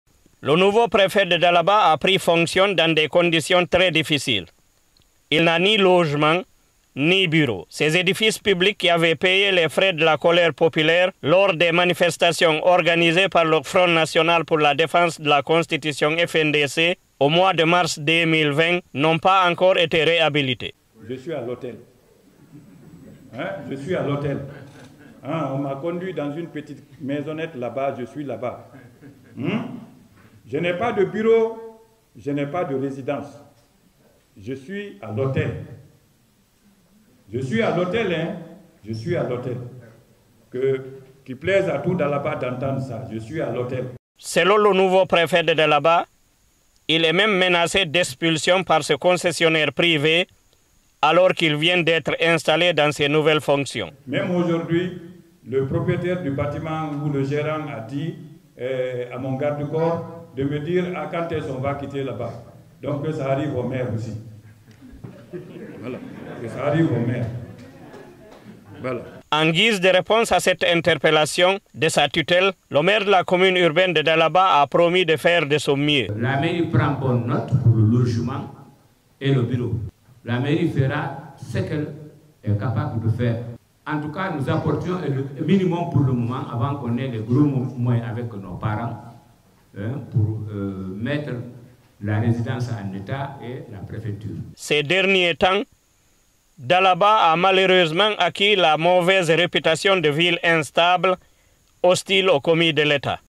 Dalaba : le nouveau préfet menacé d’expulsion par un concessionnaire privé
Le nouveau préfet de Dalaba Elhadj Boubacar Barry déclare être menacé d’expulsion de l’hôtel où il a été logé après son installation officielle. Sa résidence de fonction et le bloc administratif préfectoral avaient été vandalisés lors des manifestations organisées au mois de mars 2020 par le Front National pour la Défense de la Constitution (FNDC). Ils n’ont tours pas été réhabilités, rapporte un correspondant de Guineematin.com en Moyenne Guinée.

Le nouveau préfet de Dalaba, Elhadj Boubacar Barry a pris fonctions, dans des conditions très difficiles. Il n’a ni logement, ni bureau. Ces édifices publics qui avaient payé les frais de la colère populaire lors des manifestations organisées par le Front National pour la Défense de la Constitution (FNDC), au mois de mars 2020.
« Je suis à l’hôtel. On m’a conduit dans une petite maisonnette là-bas. Je suis là-bas. Je n’ai pas de bureau, je n’ai pas de résidence. Qu’il plaise à tout Dalaba d’entende çà. Je suis à l’hôtel » a regretté Elhadj Boubacar Barry, nouveau préfet de DALABA.
Selon le nouveau préfet de Dalaba, il est même menacé d’expulsion par ce concessionnaire privé alors qu’il vient d’être installé dans ses nouvelles fonctions.
« Même aujourd’hui, le propriétaire du bâtiment ou le gérant a dit à mon garde-corps de me demander quand est-ce on va quitter là-bas. Que ça arrive au maire aussi » a-t-il ajouté.
En guise de réponse à cette interpellation de sa tutelle, le maire de la commune urbaine de Dalaba a promis de faire de son mieux.
« La Mairie prend bonne note pour le logement et le bureau. La Mairie fera ce qu’elle est capable de faire. En tout nous apporterons le minimum pour le moment avant qu’on ait des gros moyens avec nos parents pour mettre la résidence et la préfecture en état » a promis Elhadj Ibrahima Diallo, Maire de la commune urbaine de Dalaba.
Ces derniers temps, Dalaba a malheureusement acquis la fâcheuse réputation de ville instable, hostile aux commis de l’Etat